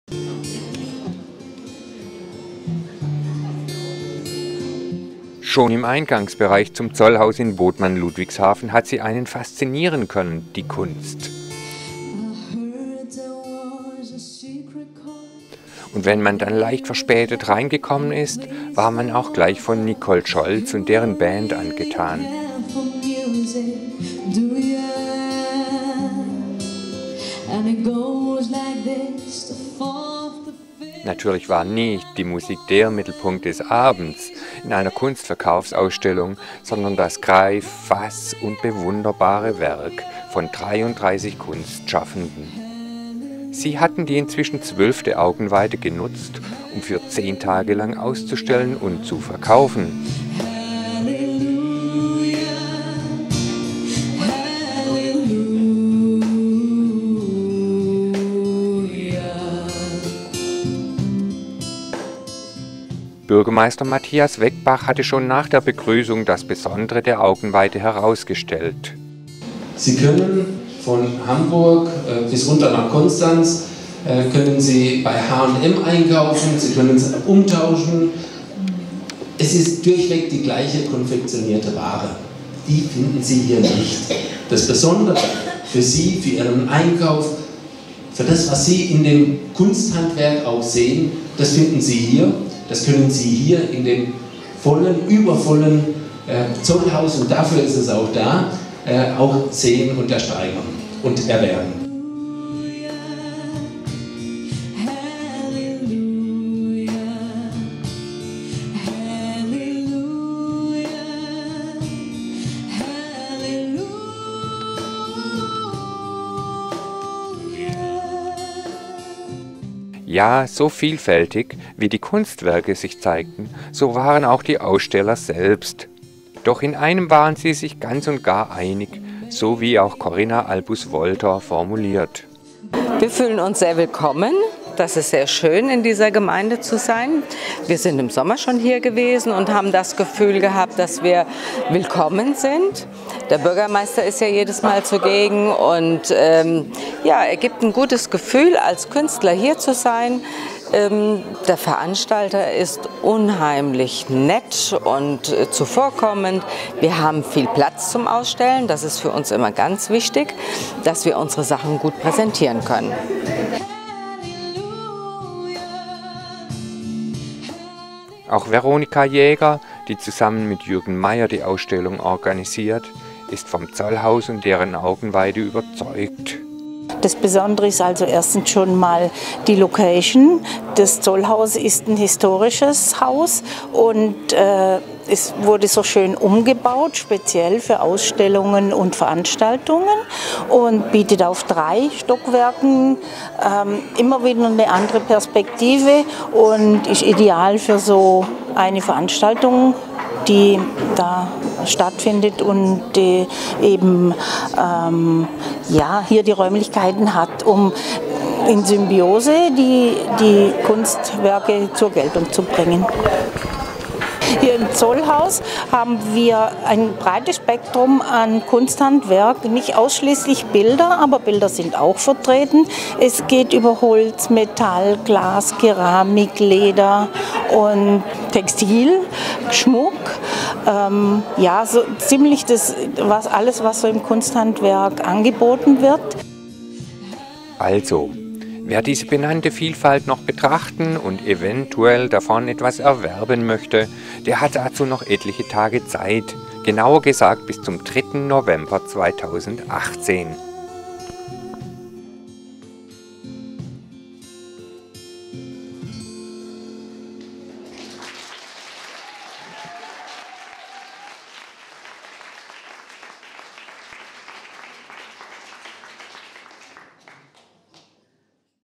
Galerie Augenweide 2018
Die nunmehr 12. Galerie Augenweide findet vom 25. Oktober bis 3. November 2018 im Zollhaus in Bodman-Ludwigshafen statt. Im Filmbeitrag sind Ein- und Ausdrücke der Vernissage dazu zu sehen.